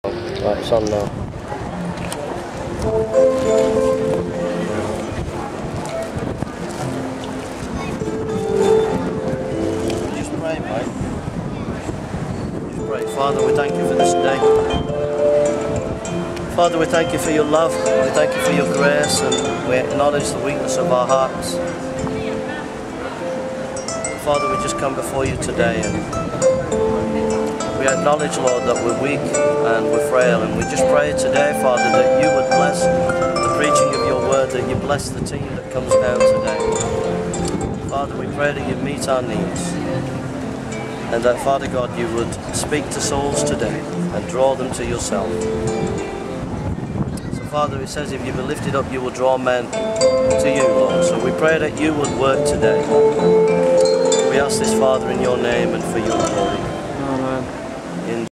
Jason Praying before he street preaches
Manchetser street preachers